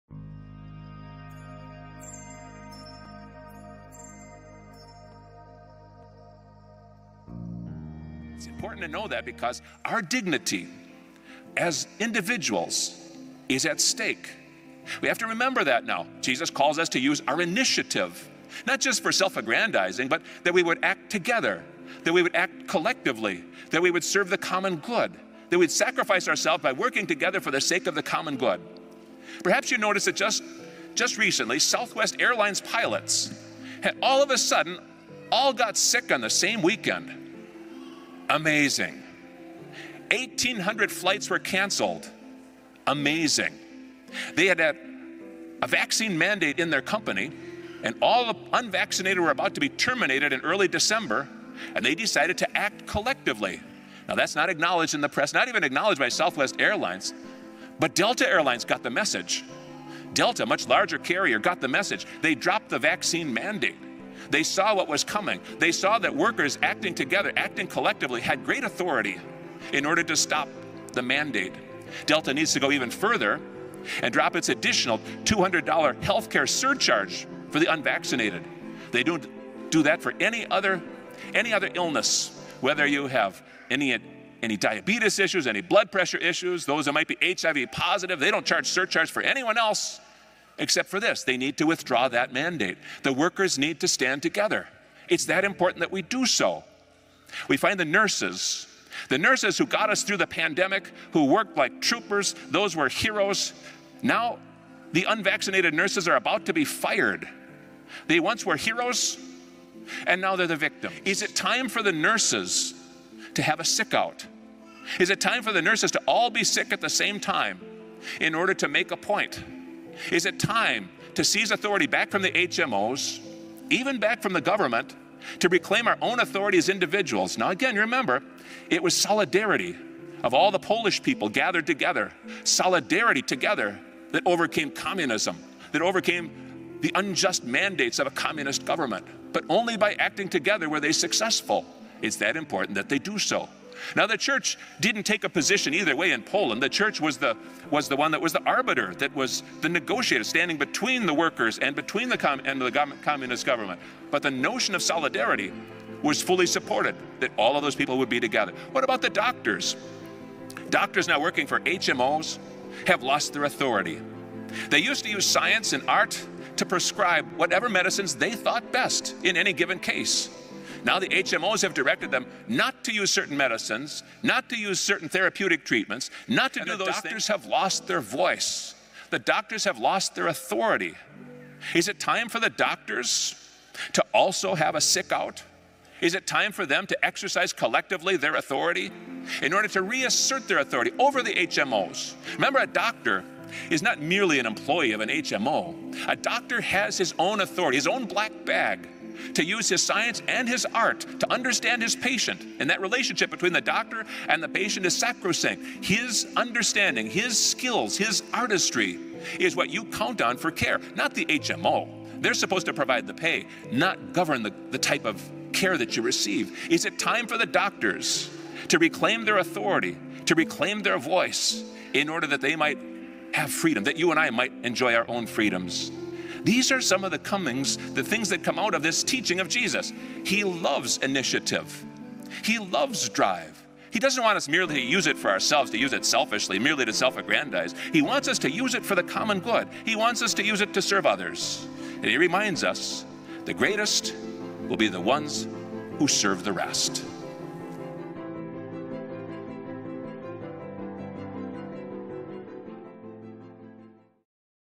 Minnesota Priest Blasts Vaxs Mandates! Urges Nurses & Doctor To Take A Stand!
Powerful sermon by priest asks the question to workers: Is it time we stand together against injustice and say no? Is it time we regain our authority and voice from tyranny?

Homily by Fr Thomas Dufner of Minnesota October 17.